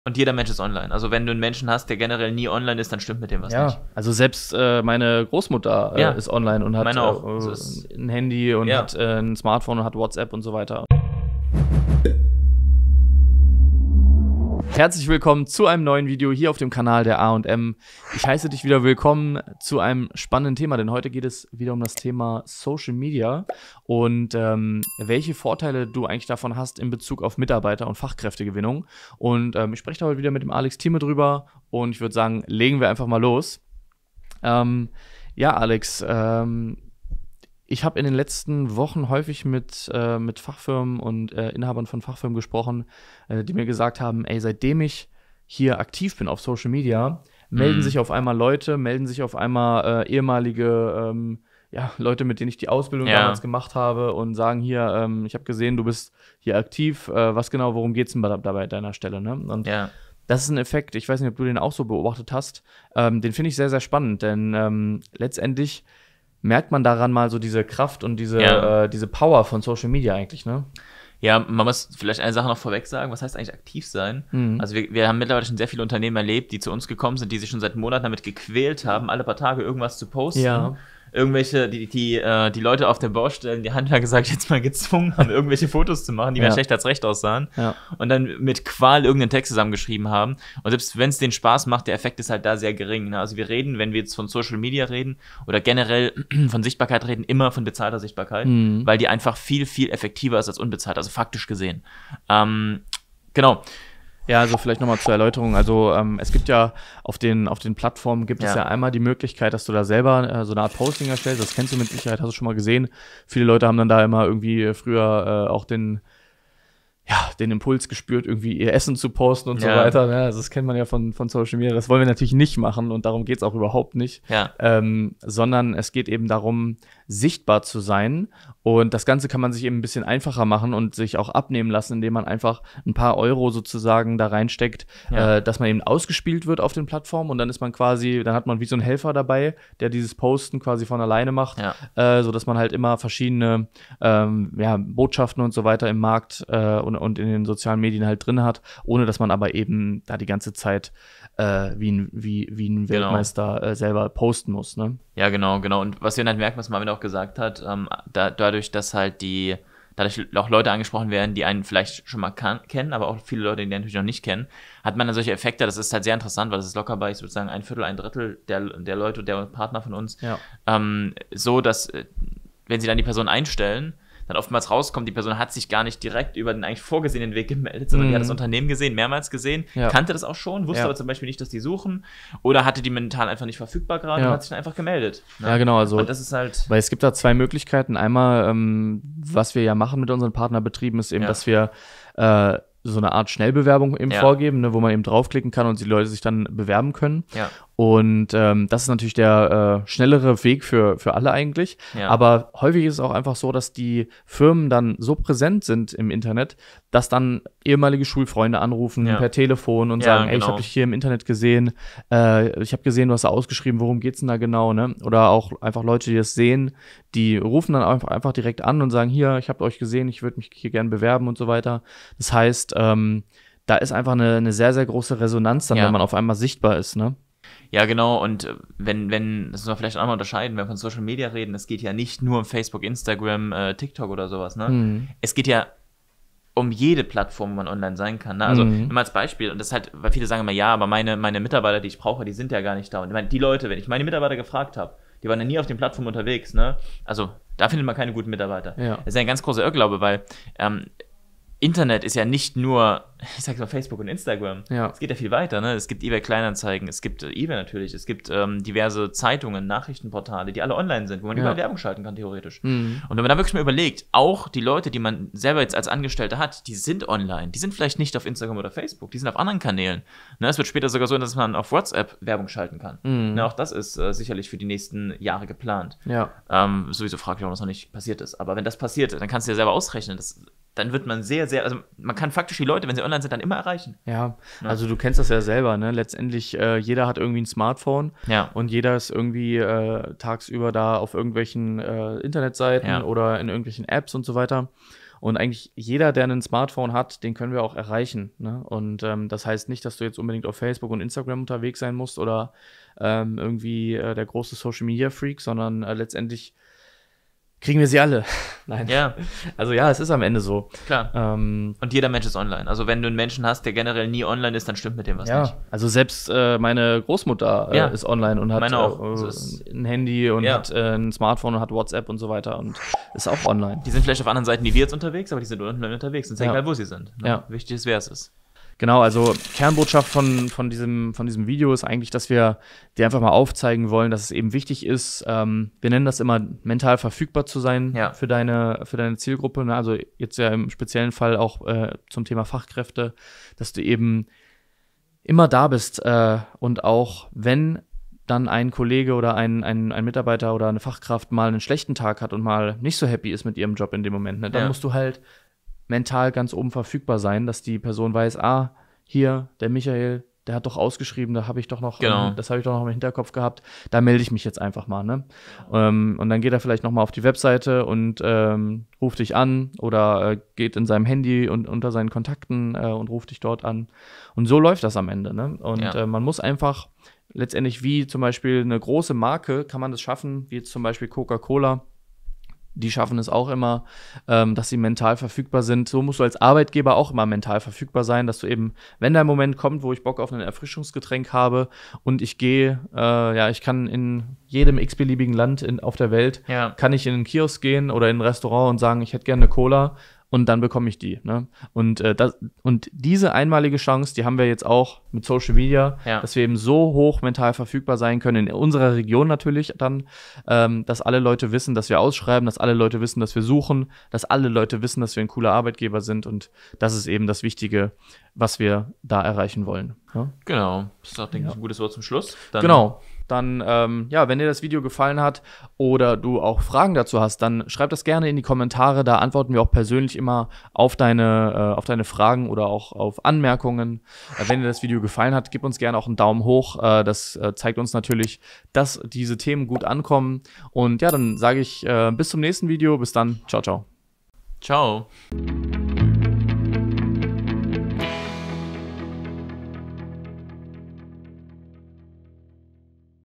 Wie du als Handwerker Fachkräfte über Social Media gewinnst! | A&M Unternehmerberatung
So findest du als Handwerker einer Fachfirma, sehr schnell gute und neue Mitarbeiter über Social Media Werbung.